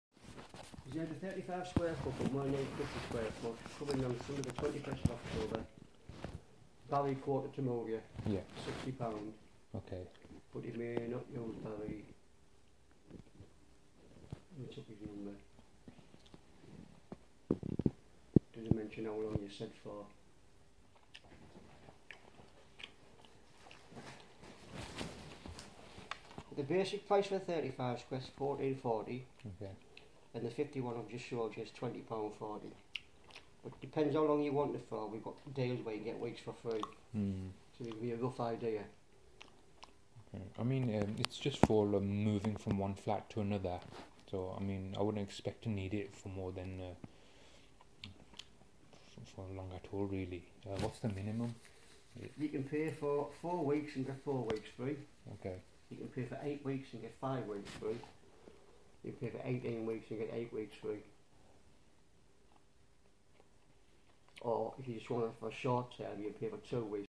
Storage King, the scammers of Wednesbury. DON'T TRUST THEM!! RESERVATION FEE SCAM
ACCURSED SCAMMERS!
Beware of the dog Lindsay! Troublesome scammer!
Untrustworthy Rodents! Preying on the sheep of Walsall and Wednesbury.

THEY ARE LIKE CRACK ADDICTS. THEY BECAME DISHONEST THIEVES FOR £20!!!

Storage King, also known as Select Storage. Station Street, Walsall, WS10 8BW.

Uploaded by JATT BAWA